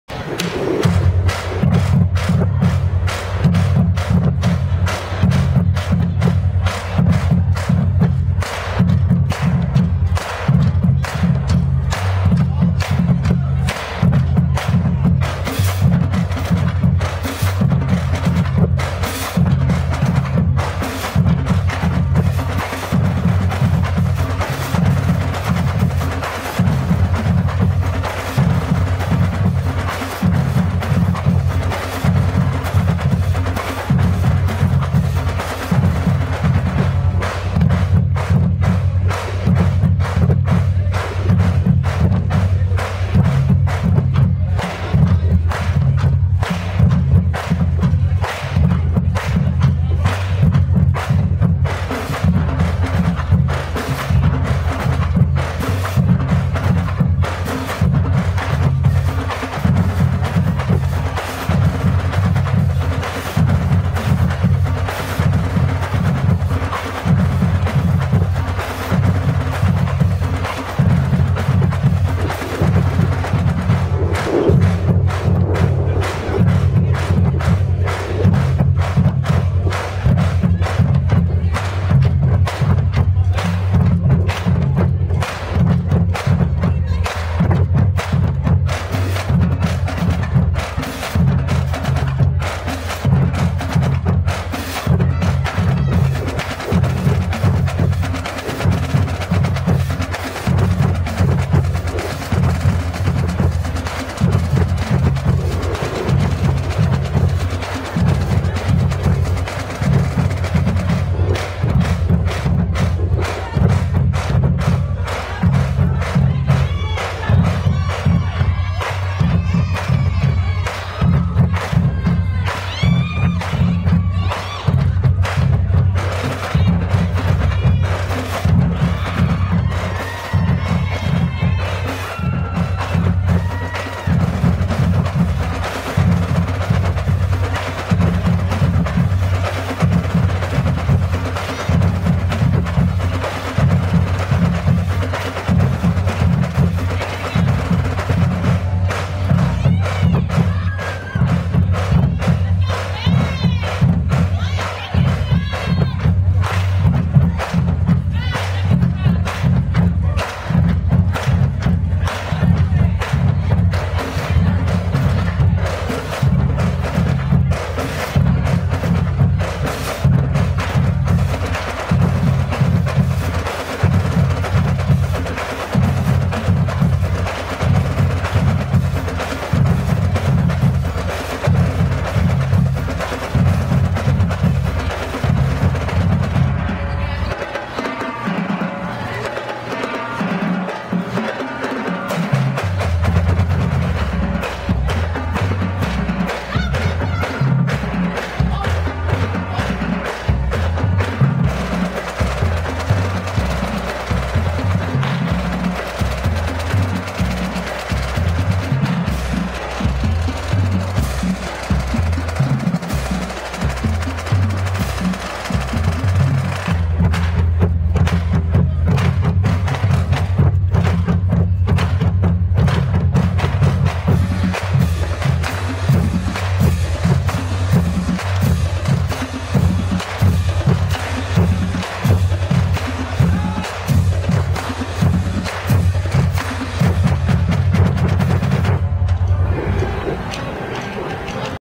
The John Jay Mustang Band performs at the Jay vs East Central football game on Friday, August 31, 2012.